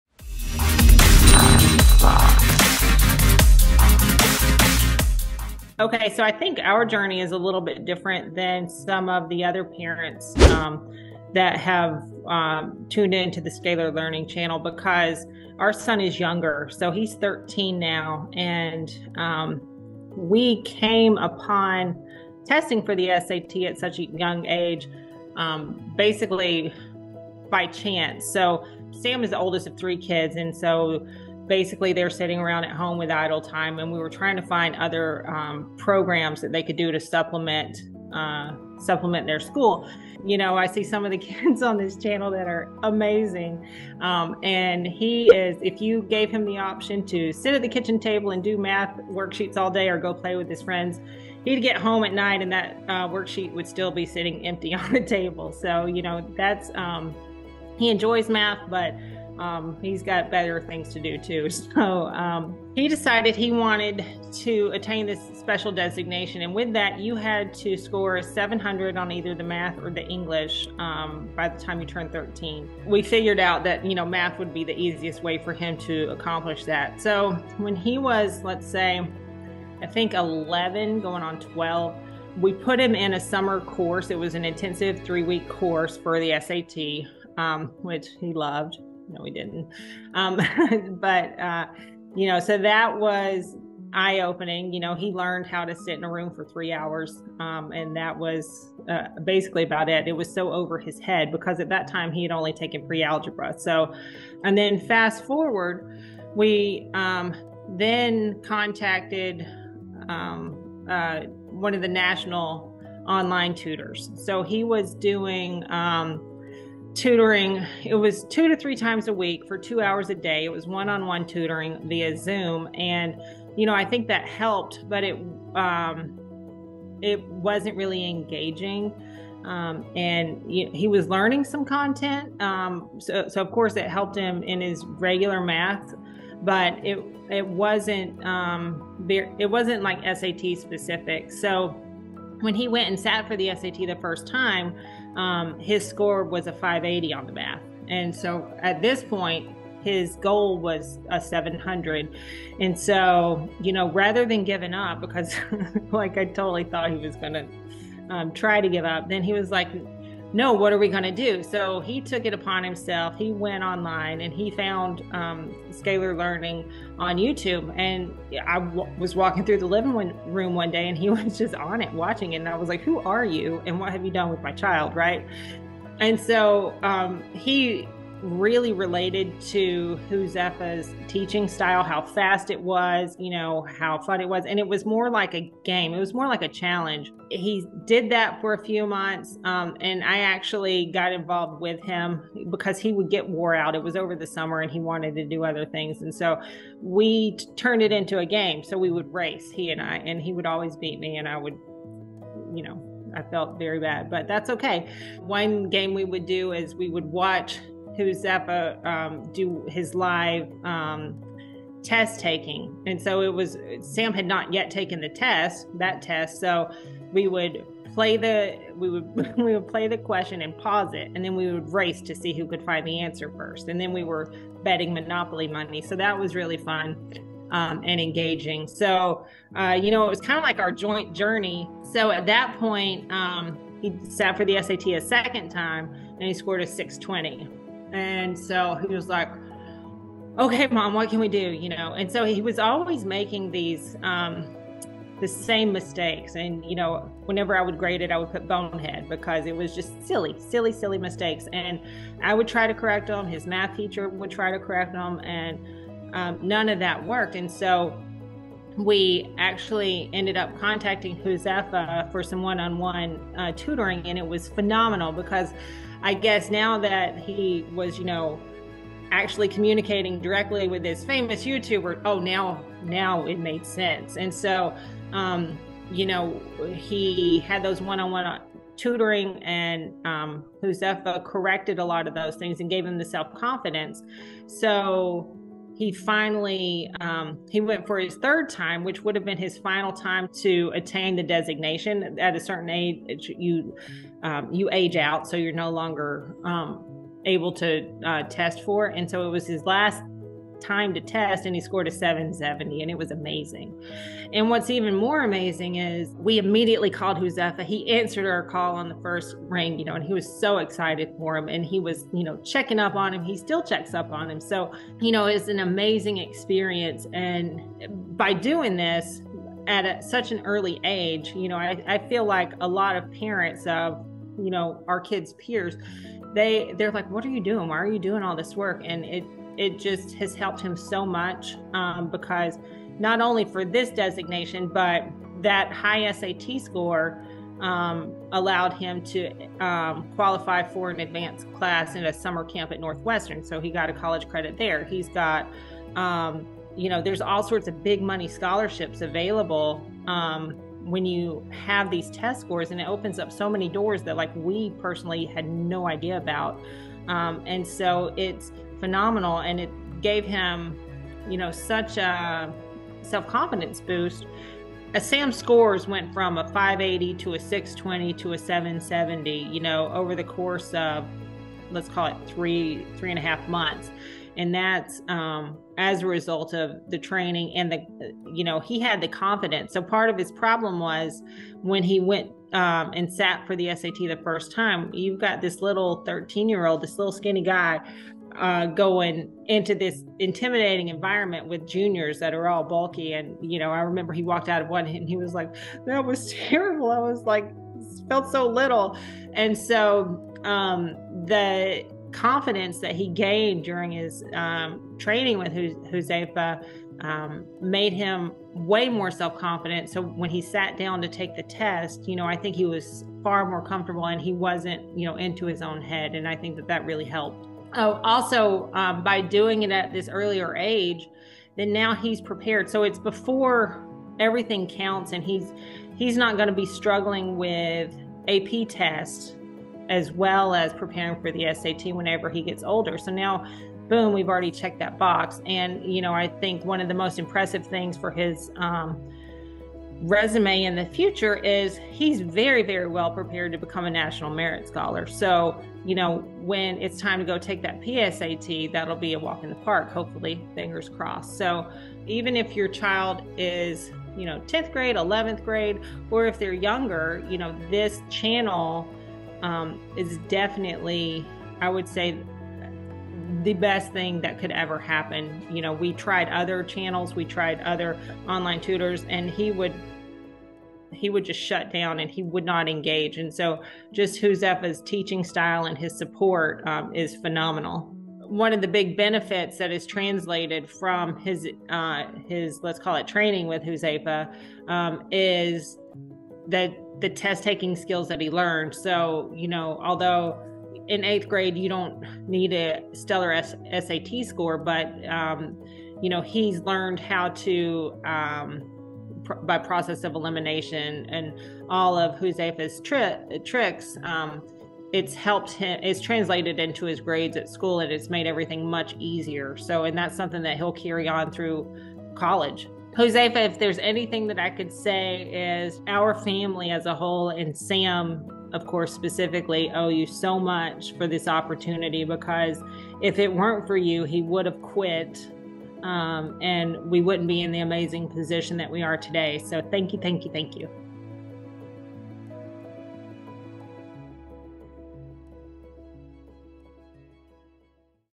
Testimonial - Working with Huzefa for SAT Math Prep (Alisa)
Huzefa has worked with hundreds upon hundreds of students in mathematics since the inception of Scalar Learning. In this video, Alisa, a cherished Scalar Learning client, tells her story of what it was like to have her son, Sam, work with Huzefa to prepare for the SAT. Alisa also describes all of the tangential benefits her son has received as a result of scoring a 770 on the math portion of the SAT!

00:00 Introduction
00:28 Why Alisa began looking for programs for her son during the pandemic
01:02 Why Sam decided to take the SAT in middle school
01:22 What is was like working with different companies and tutors
02:39 Finding the Scalar Learning channel after taking an initial SAT
03:51 Sitting for the SAT a second time
04:18 Hiring Huzefa for one-on-one tutoring
04:28 Why working with Huzefa was different
05:00 Achieving a 770 on the SAT math during the third attempt
05:17 The benefits of achieving this score on the SAT
06:04 Confidence gained throughout the test prep process
07:31 What's special about Huzefa's teaching style
07:57 Test taking skills translating to school academics
08:32 A final message from Alisa to Huzefa

Need academic help? Learn more about the Scalar Learning Tutoring Team